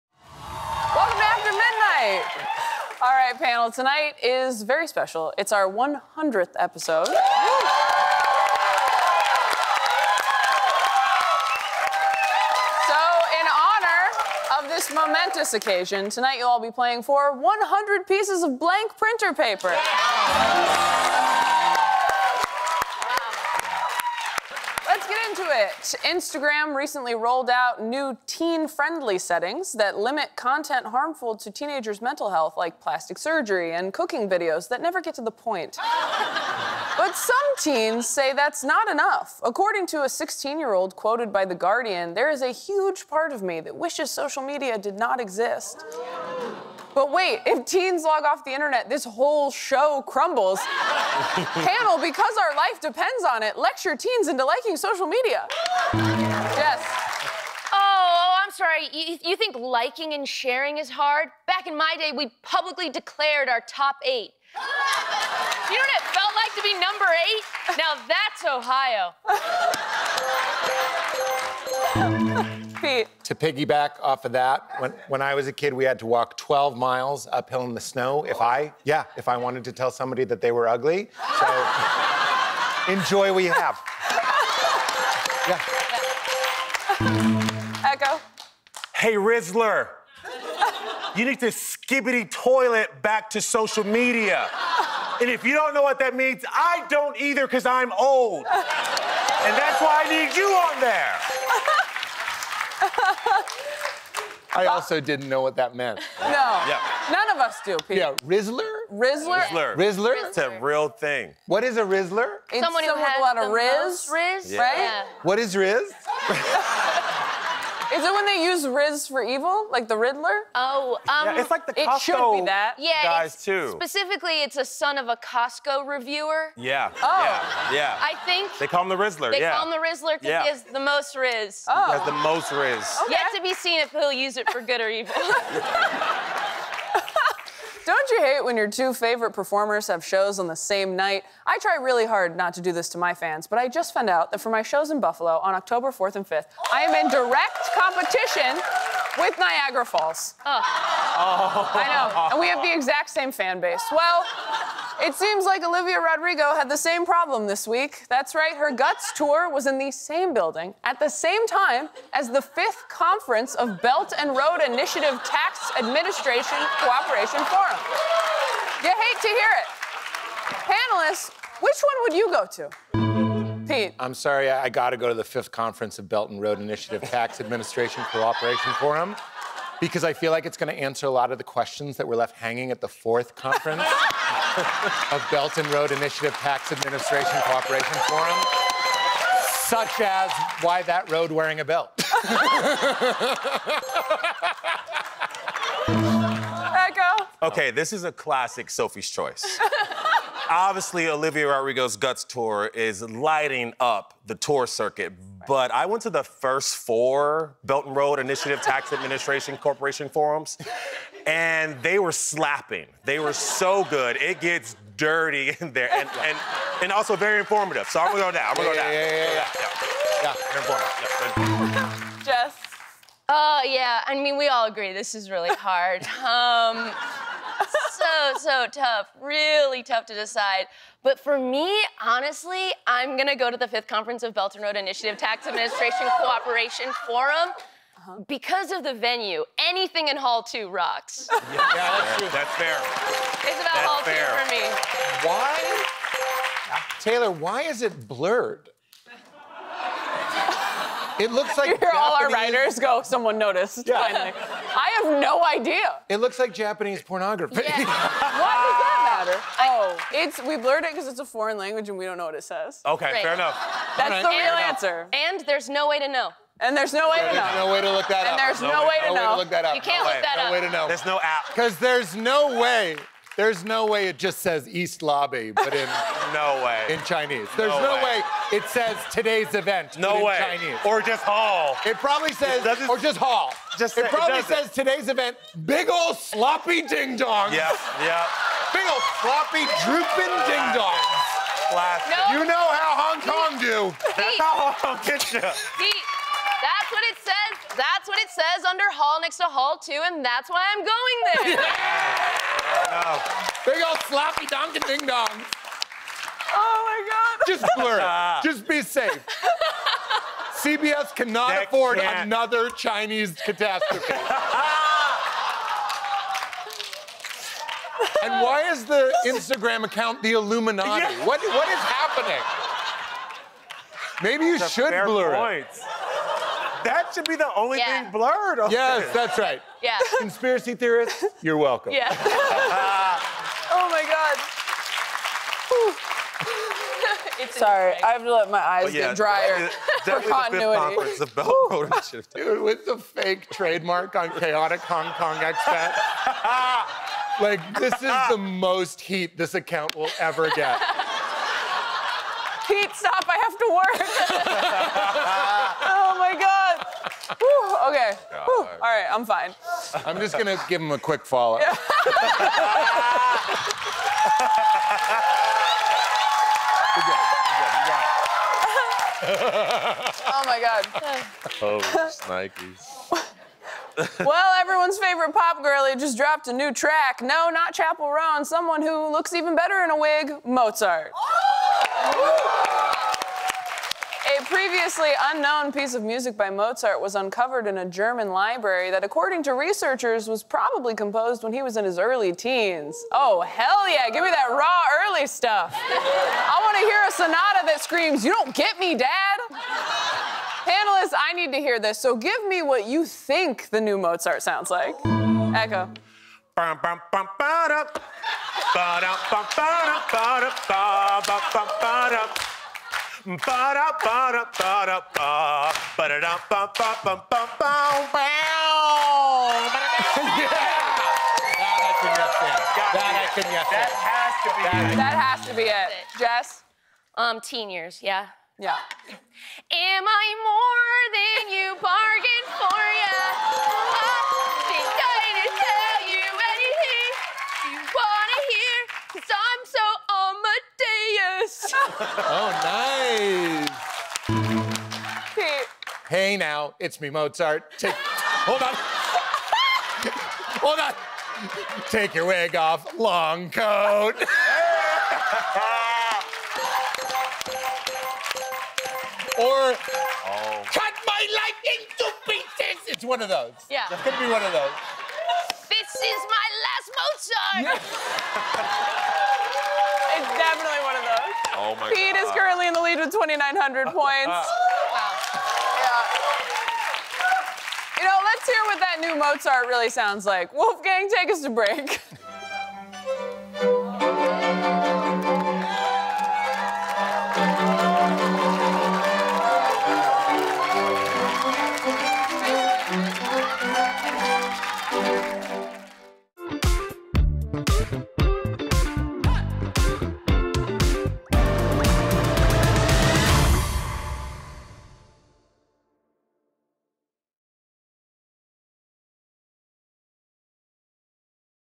Pete Holmes Has Some Questions About Blurred Olivia Rodrigo Sign
Taylor Tomlinson invites Jessica McKenna, Pete Holmes, and Echo Kellum into the group chat! They talk about what scams they’re running, Instagram for teens, a subscription app for phone wallpapers, and a blurred sign that everyone has some questions about.